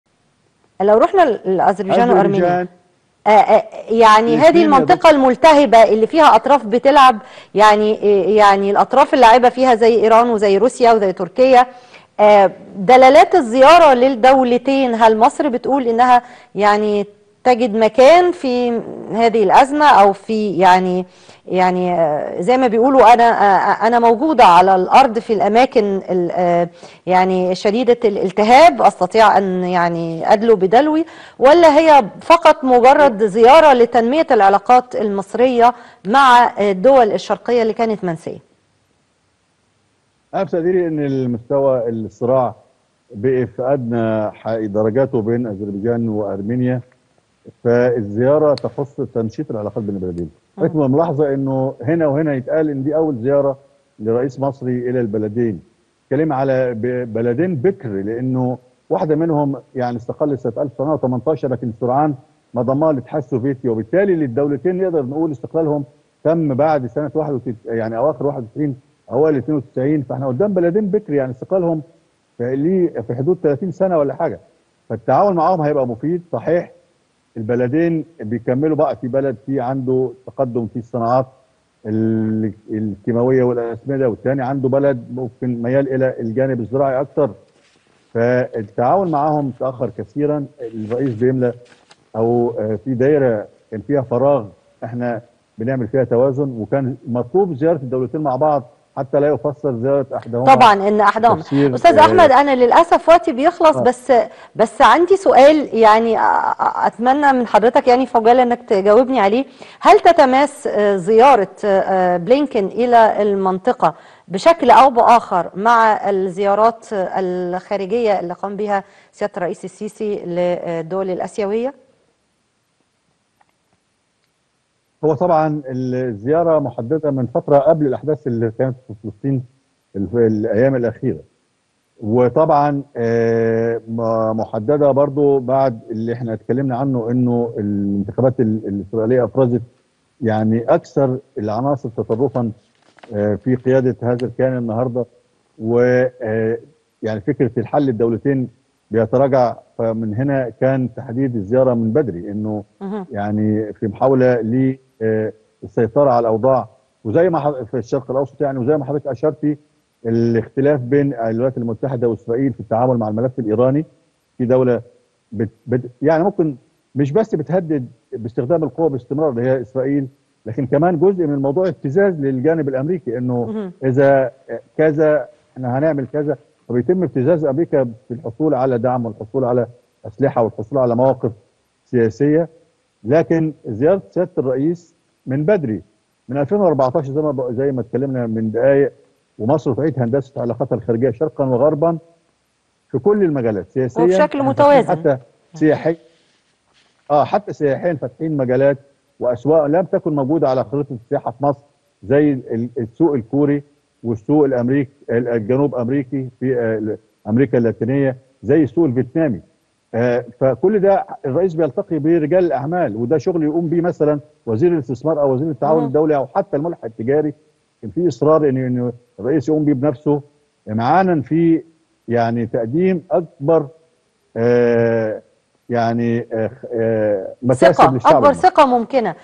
الكاتب السياسي أحمد رفعت يحلل دلالات زيارة الرئيس السيسي إلى أذربيچان وأرمينيا.
الكاتب السياسي أحمد رفعت
يحلل دلالات زيارة الرئيس السيسي إلى أذربيچان وأرمينيا.

👈 يمكنكم الآن مشاهدة قناة Mesat
على التردد
11603 | ١١٦٠٣
Horizontal | أفقي
27500 | ٢٧٥٠٠
3/4 | ٣/٤